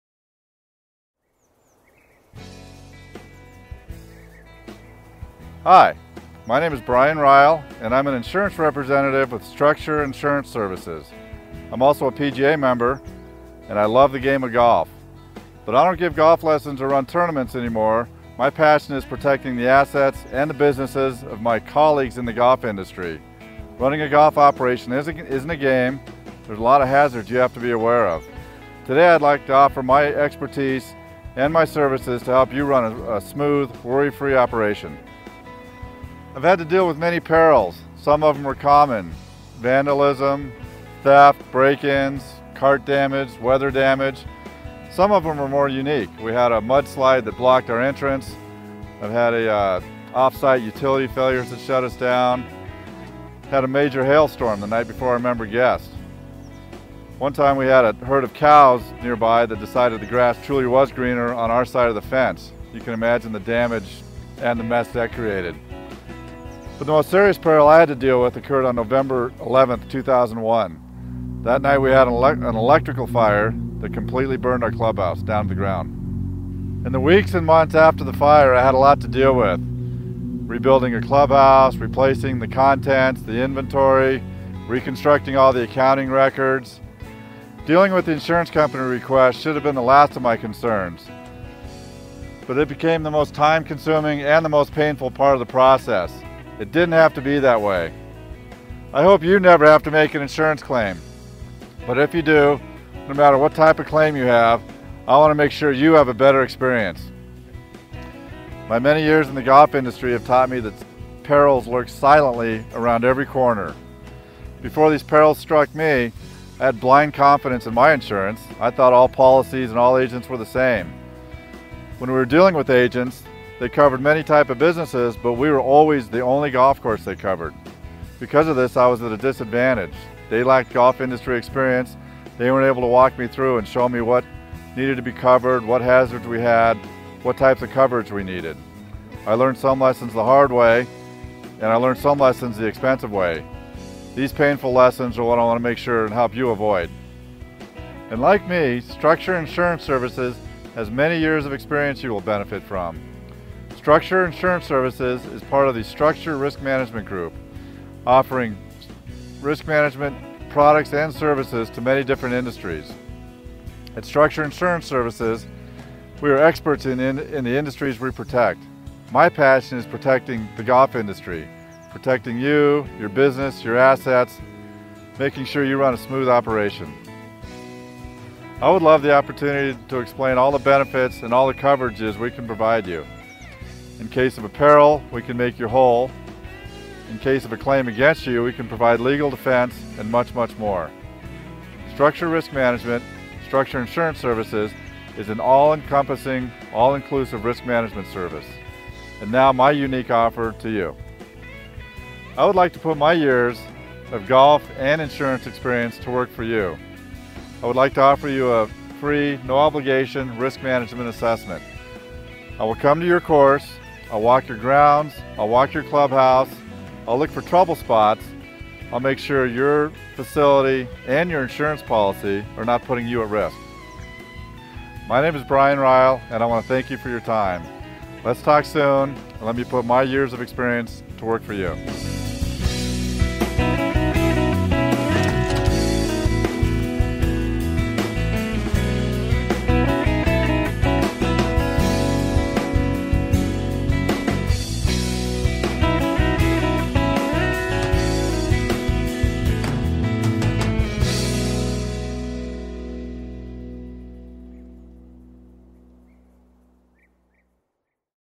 StrucSure Insurance Services - Golf Course Coverage
Brian Ryall, an insurance representative of StrucSure Insurance Services, brings his years of experience in golf course management and insurance services brings you a special message and unique offer in this video.